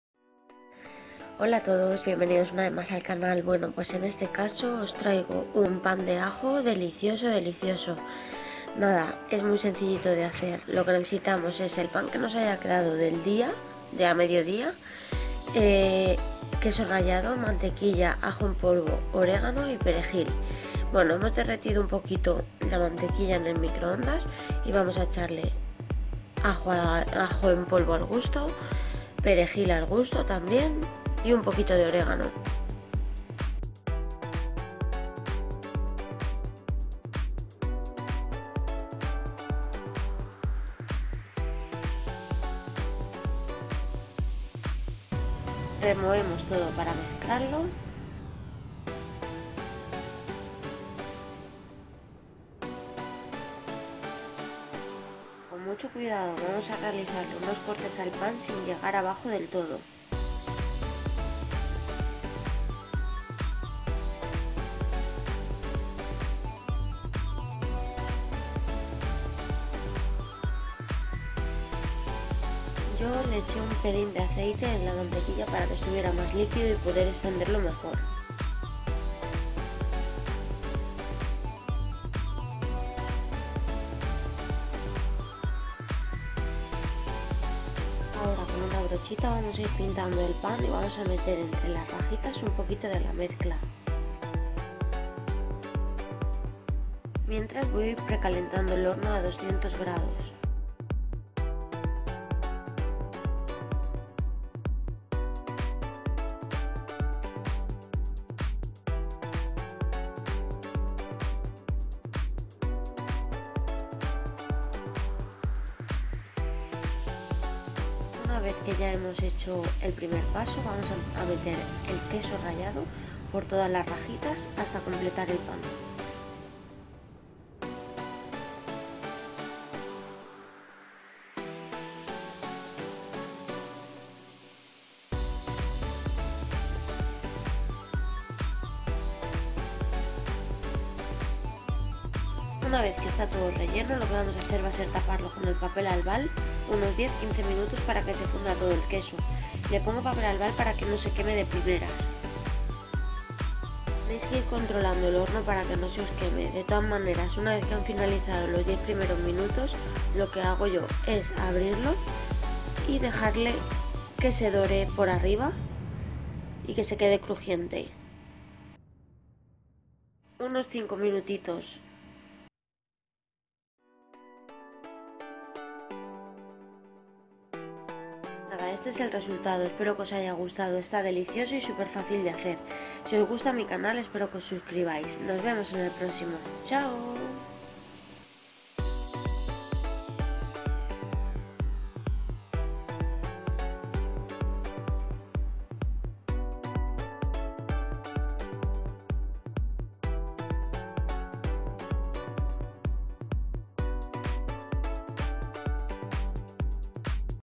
PAN DE AJO CON QUESO FACIL Y RÁPIDO#
Hola a todos bienvenidos una vez más al canal ! Hoy os traigo un pan de ajo delicioso y mi fácil y rápido!!

*Soy la creadora 100% original del contenido de mis vídeos de este mi canal *